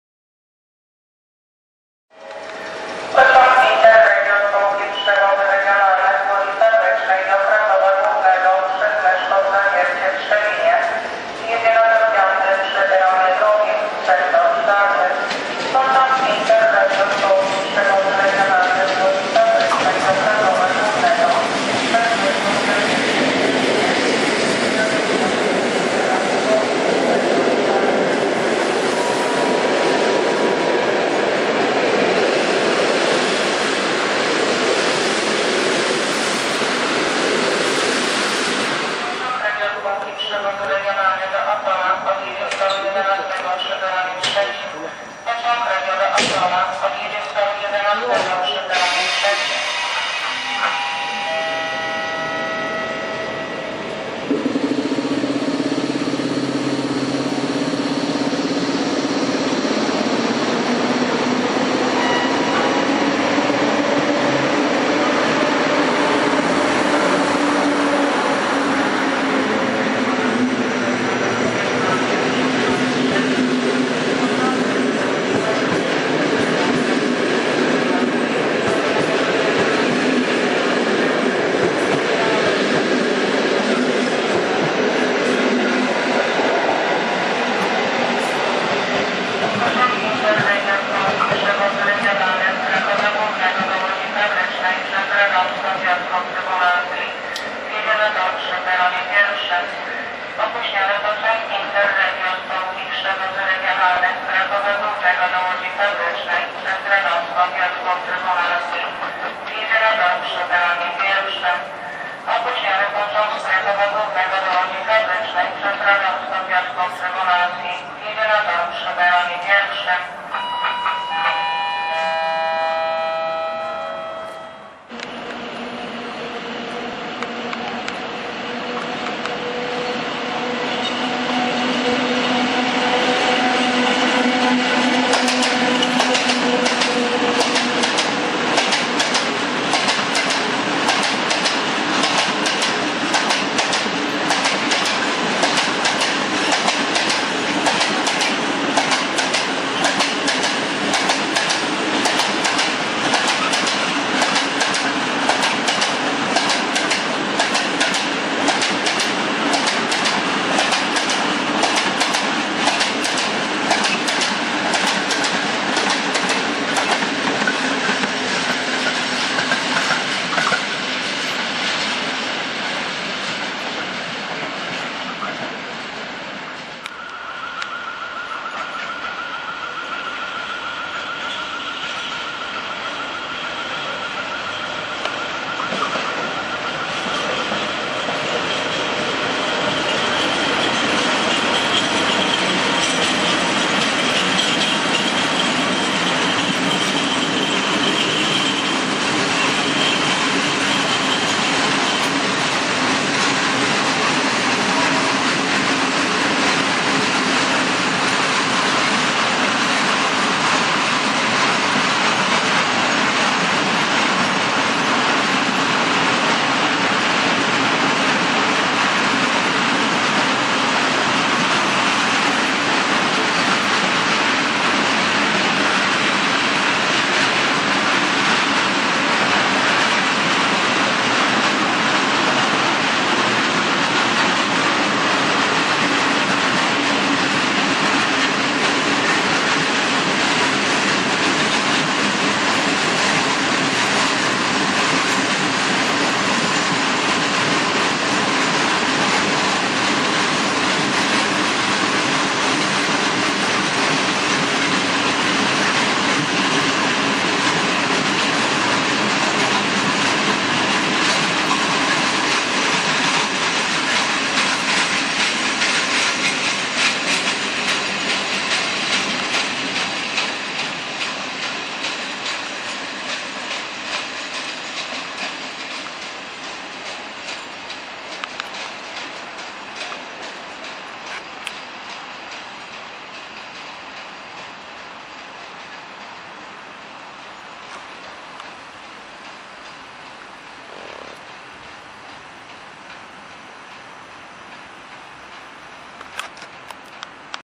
EU07-326, ET22-551 oraz ET22-1085 [Częstochowa Osobowa]
Wjazd i odjazd pociągu spółki PKP ICC TLK (Tanie Linie Kolejowe) 41104 relacji: Katowice - Warszawa Wschodnia. Następnie przejazd ET22-551 kierującego się w stronę Radomska oraz przejazd ET22-1085 kierującego się w stronę Zawiercia. Składy nagrano: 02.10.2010r. Życzę miłego oglądania.